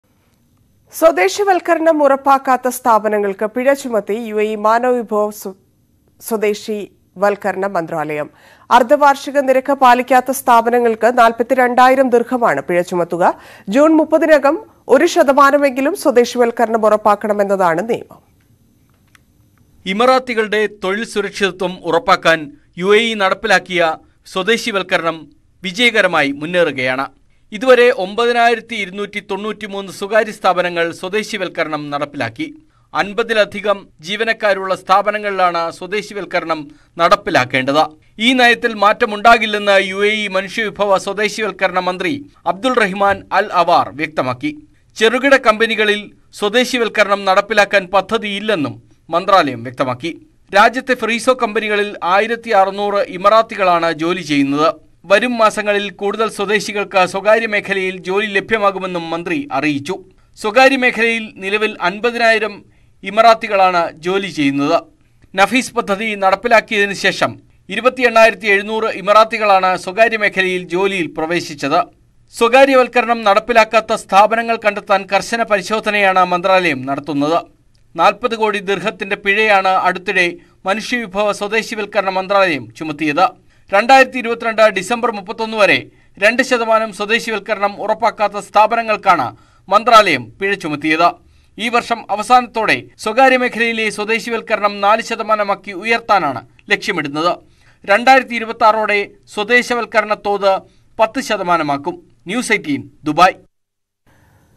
Gulf18 | സ്വദേശിവല്‍ക്കരണം ഉറപ്പാക്കാത്ത സ്ഥാപനങ്ങള്‍ക്ക് പിഴ ചുമത്തി UAE | Malayalam News Today
Gulf18 : സ്വദേശിവല്‍ക്കരണം ഉറപ്പാക്കാത്ത സ്ഥാപനങ്ങള്‍ക്ക് പിഴ ചുമത്തി UAE.
നടപടിയുമായി മാനവ വിഭവ സ്വദേശിവല്‍ക്കരണ മന്ത്രാലയം. ഒരു മാസത്തിനകം ഒരുശതമാനം സ്വദേശിവല്‍ക്കരണം ഉറപ്പാക്കണമെന്ന് നിയമം.